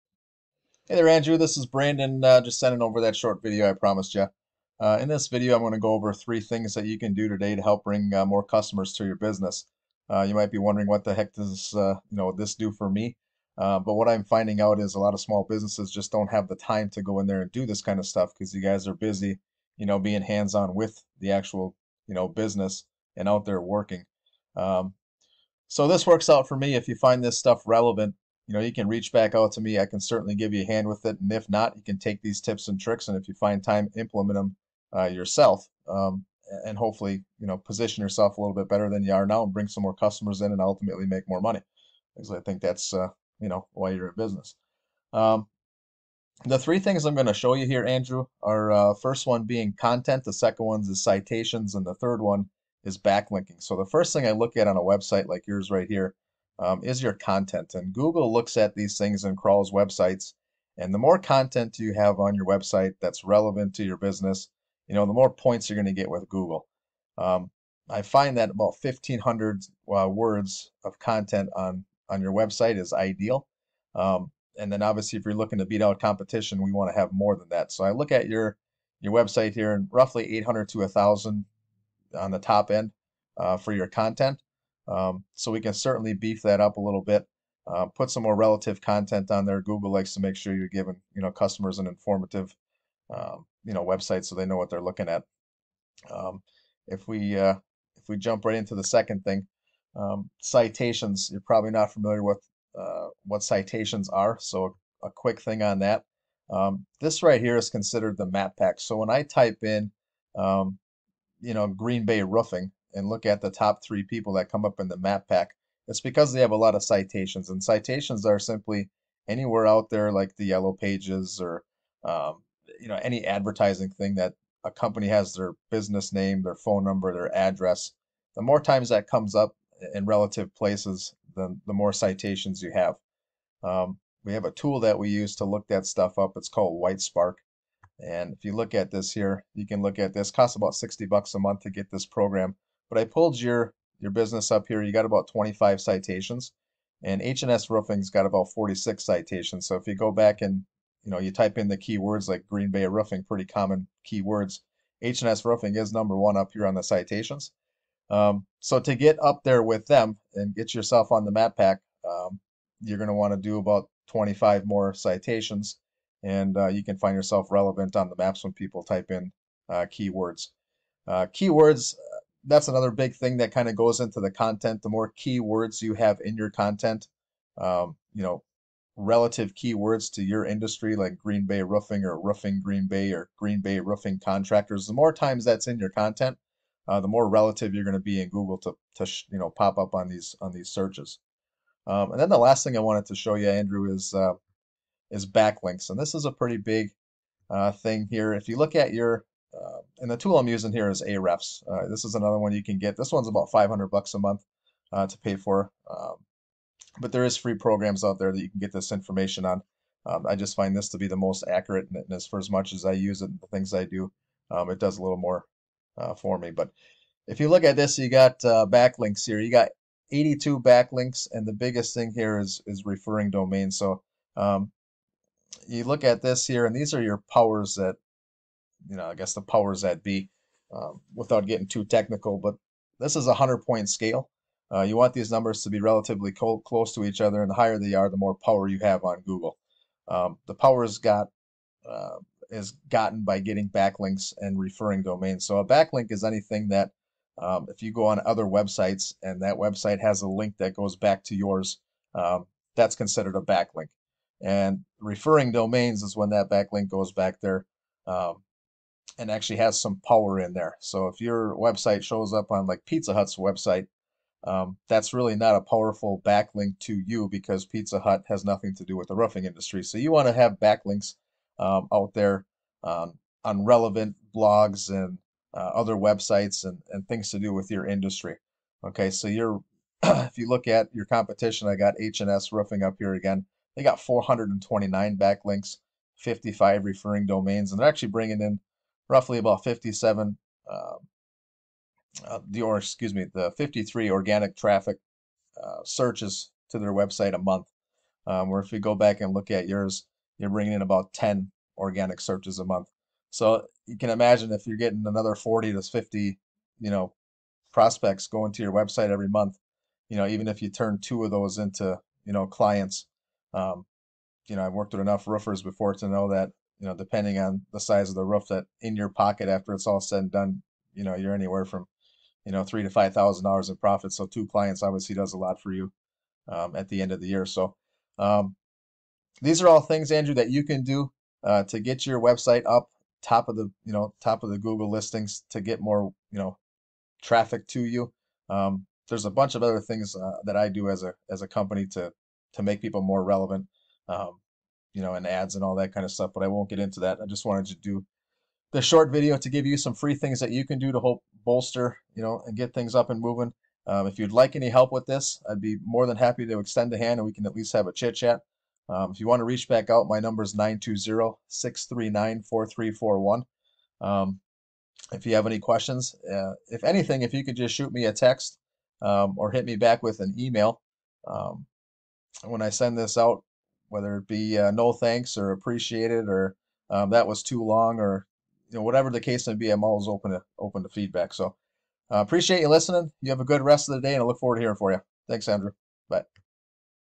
Andrew - Aim Roofing
Helping Andrew at Aim Roofing increase his website traffic.